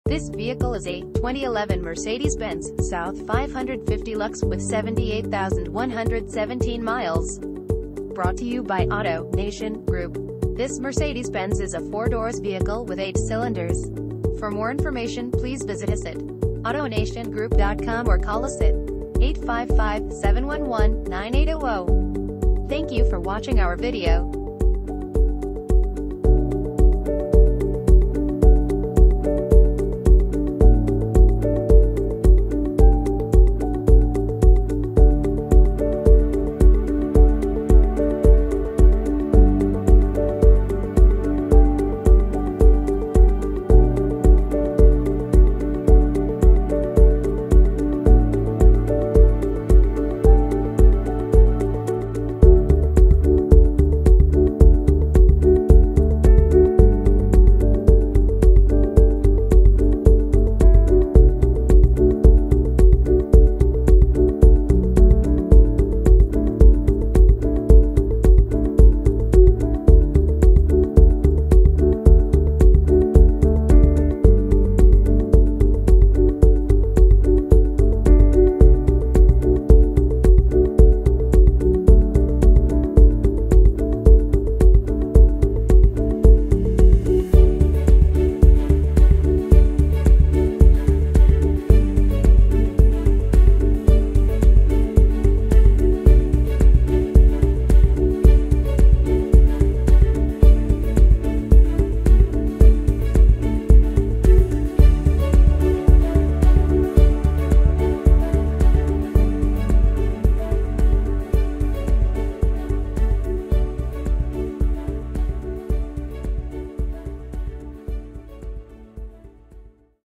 2011 Mercedes-Benz S 550 - Auto Nation Group
Auto Nation Group
21024 Sherman Way Unit F

2011 Mercedes-Benz  S 550

2 owners, Clean title, 3 Months / 3,000 Miles Powertrain, 78,117 miles, Automatic, Black exterior, Black interior, Luxury, RWD: Rear Wheel Drive, 4 doors, 8 cylinders, Gasoline, MPG City 15 and 23 in Hwy.